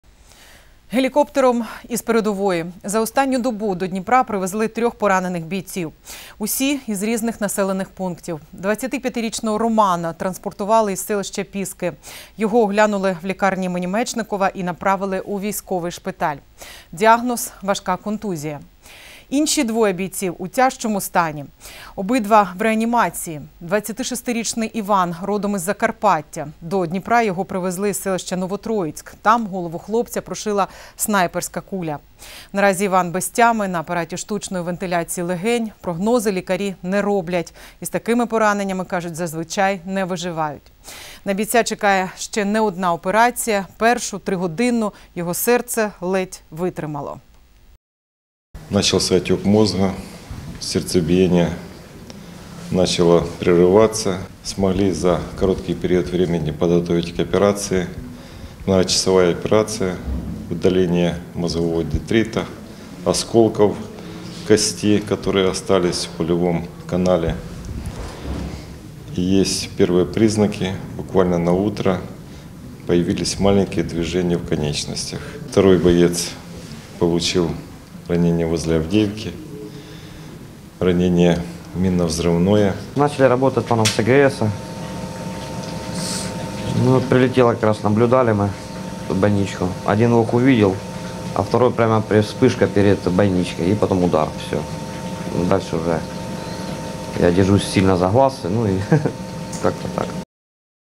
Гелікоптером з передової до Дніпра транспортували трьох поранених бійців
Вертолітом з передової. За останню добу до Дніпра привезли трьох поранених бійців. Усі - із різних населених пунктів. 25-річного Романа транспортували із селища Піски. Його оглянули в лікарні імені Мечникова і направили у військовий шпиталь. Діагноз: важка контузія. Інші двоє захисників — у тяжчому стані. Обидва — в реанімації. 26-річний Іван родом із Закарпаття. Його привезли із селища Новотроїцьке. Там голову хлопця прошила снайперська куля. Наразі Іван  без тями - на апараті штучної вентиляції легень. Прогнози лікарі не роблять: з такими пораненнями не виживають. На бійця чекає ще не одна операція. Першу - тригодинну - його серце ледь витримало.